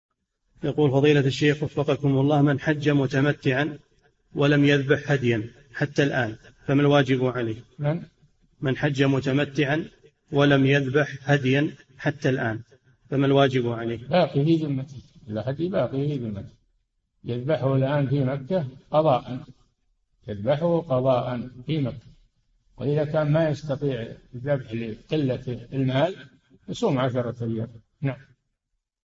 [2934] حكم من حج متمتعاً ولم يذبح هدياً - الشيخ صالح الفوزان
"[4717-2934] من حجَّ متمتعًا ولم يذبح هديًا حتى الآن فما الواجب عليه؟
الجَوَابُ: باق في ذمته، الهدي باق في ذمته، يذبحه الآن في مكة قضاء، يذبحه قضاء في مكة وإذا كان ما يستطيع الذبح لقلة المال يصوم عشرة أيام.

للاشتراك في قناة الشيخ صالح الفوزان

للاستفسار والإقتراحات والدعم
رئيس الفريق
لمشروع كبار العلماء